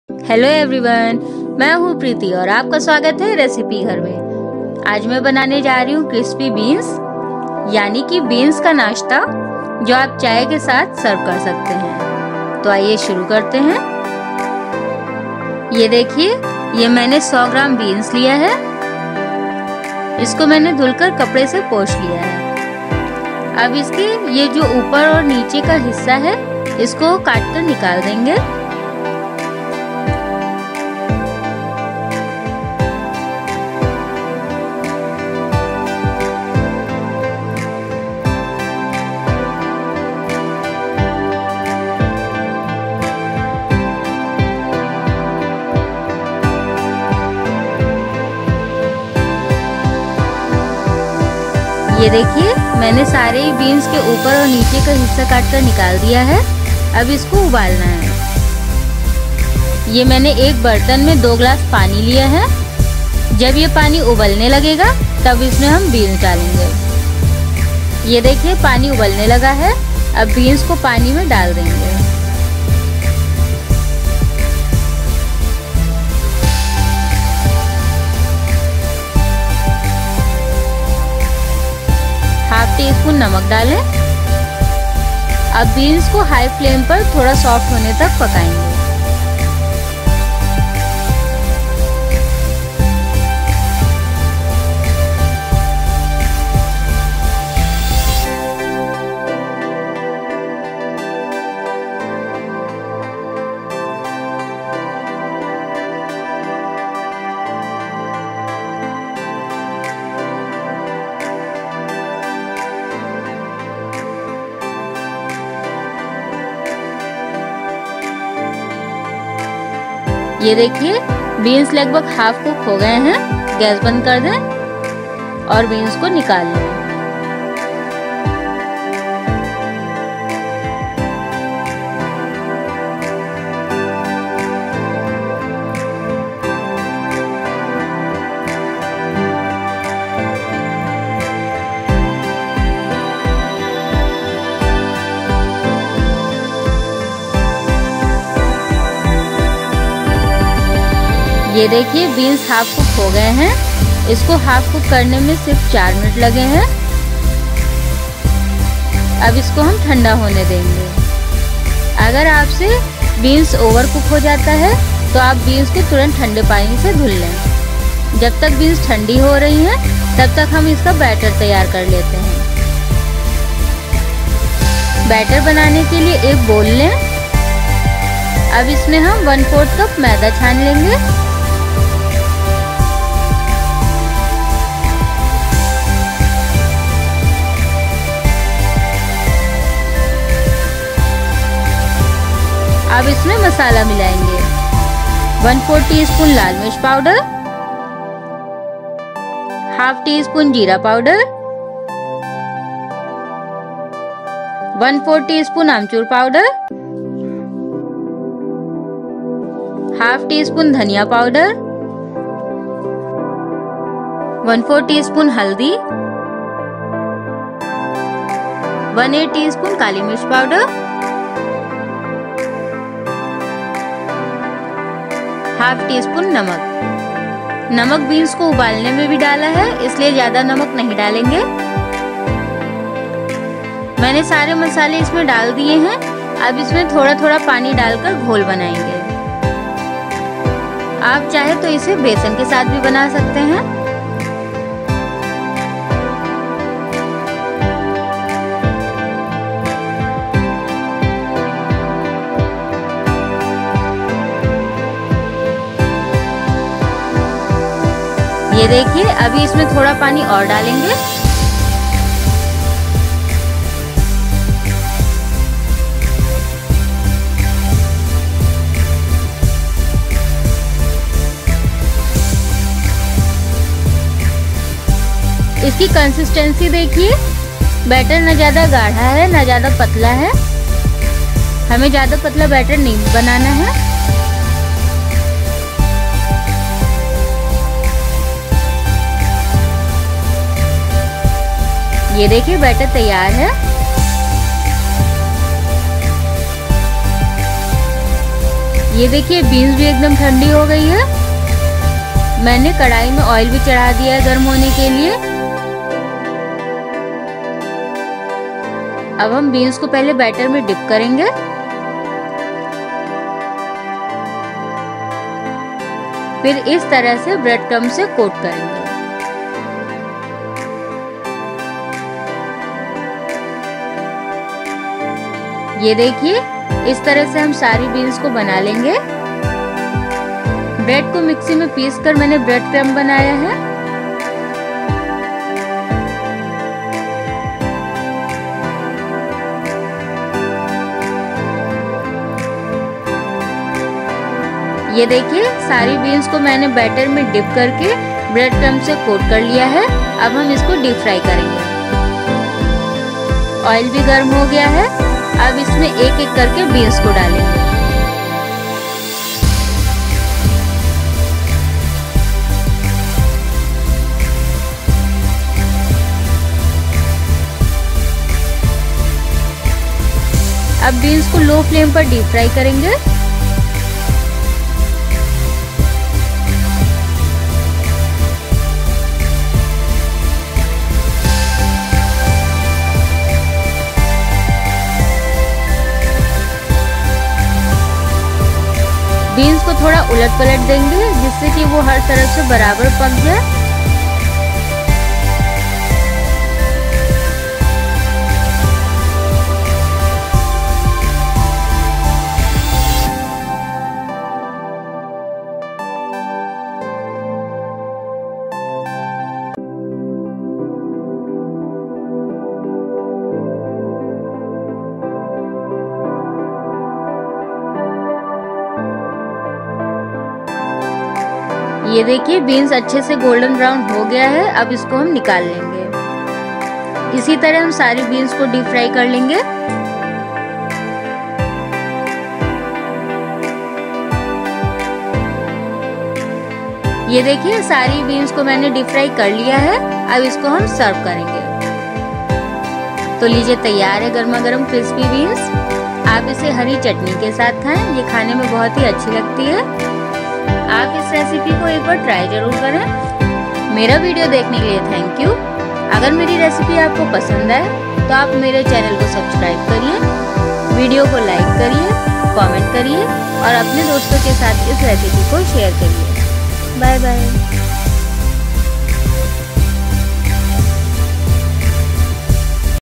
Crispy green bean recipe | Bean fry | Snack recipe | Tea time snack recipe
Hello everyone!
Today's video is about how to make Crispy green bean recipe.

Ingredients:-

100gm- green beans
¼cup- all purpose flour
¼tsp - red chili powder
½tsp - Cumin powder
¼tsp - dry mango powder
½tsp - Coriander powder
⅛tsp - Black pepper powder
¼tsp  - Turmeric powder
bread crumbs